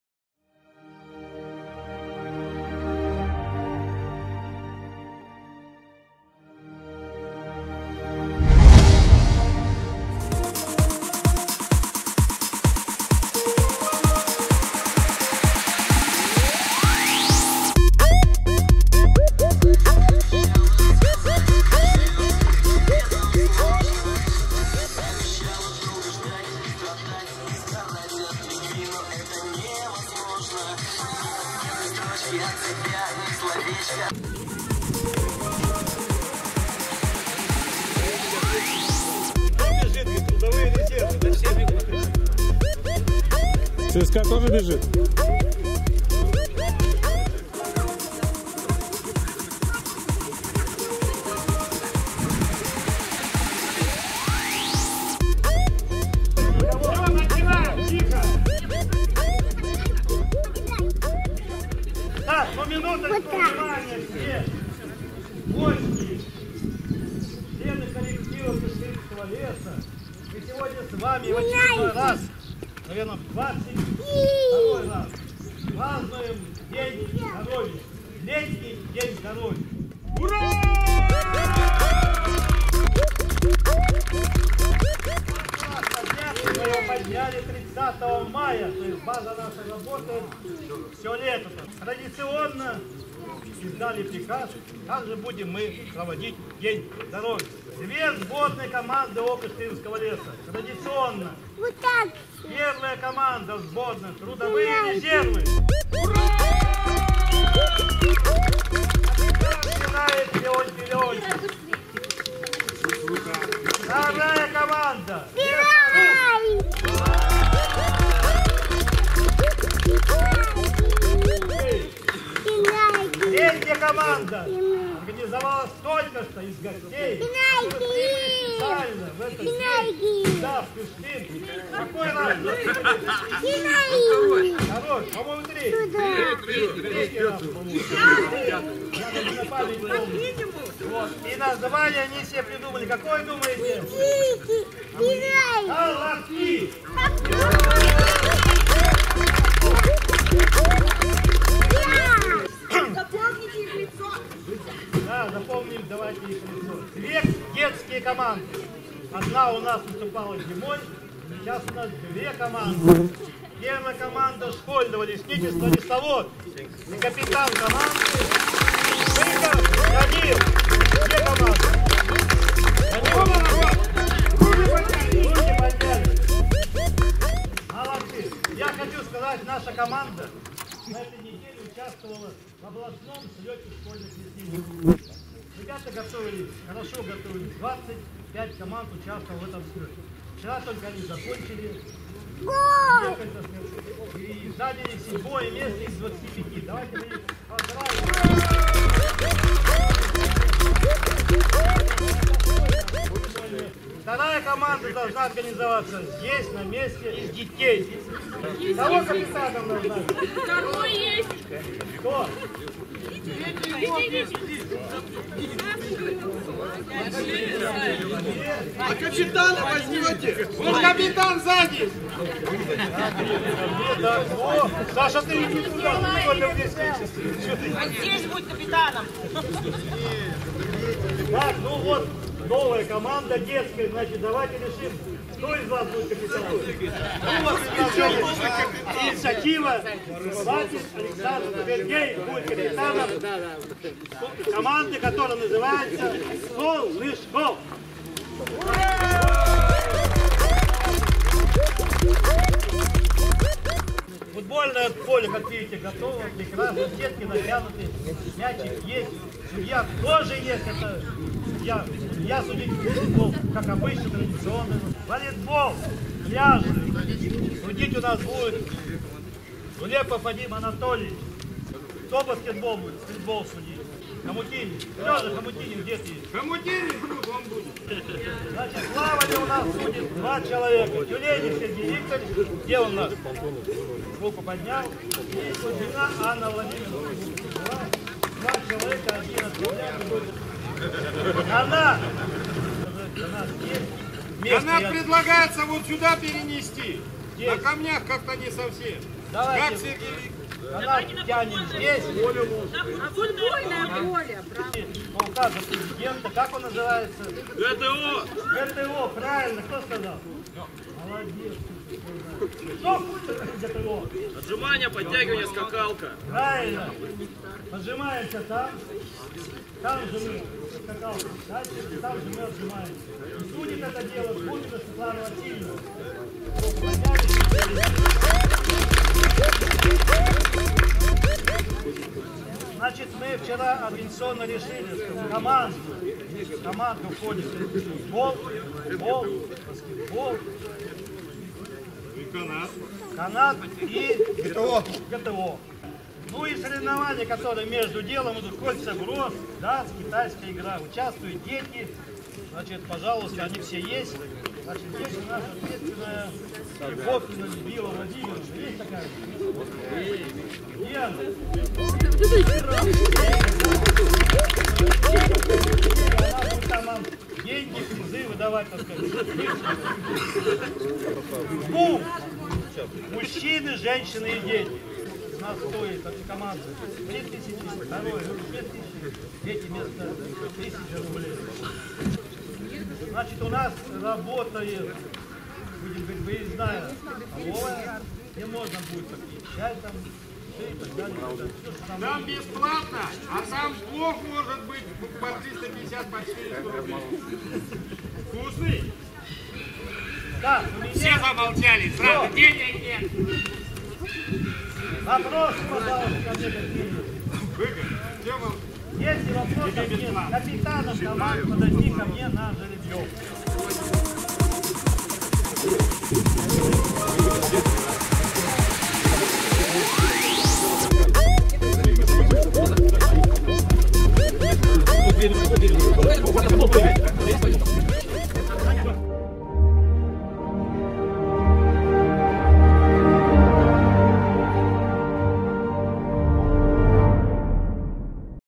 Летний день здоровья ч 1 Открытие
ООО"Кыштымский лес" провёл традиционный Летний День здоровья.
В программе мероприятия соревнования по футболу, волейболу, стритболу, ГТО, перетягиванию каната и т.д.